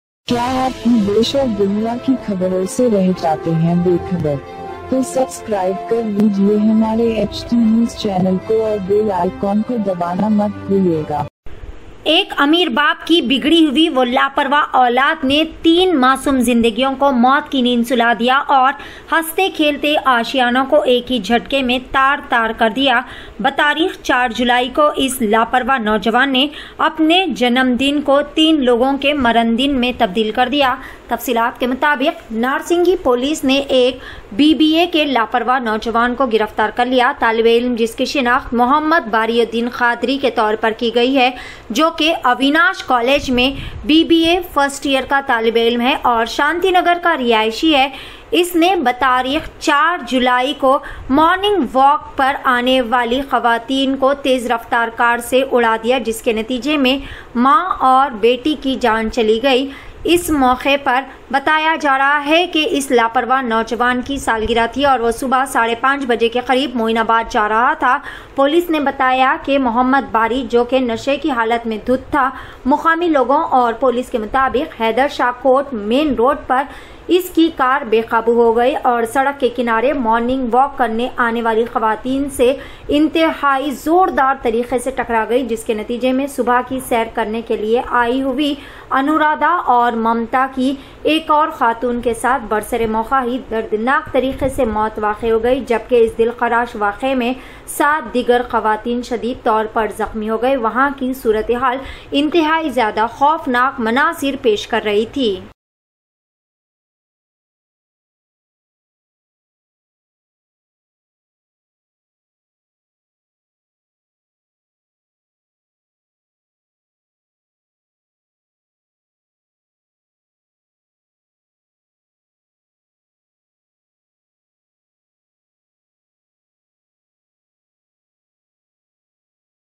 Two 19-year-olds have been arrested after a mother-daughter duo was killed in a horrific accident
Hyderabad- NarsingiPolice have arrested a BBA student- Md Badi Uddai Khadri- Studying in Avinash College
(BBA 1st year, Resident of Shanti Nagar) in the ghastly accident.

It was his birthday and they were going to Moinabad around 5:30 am. They were drunk, cops said.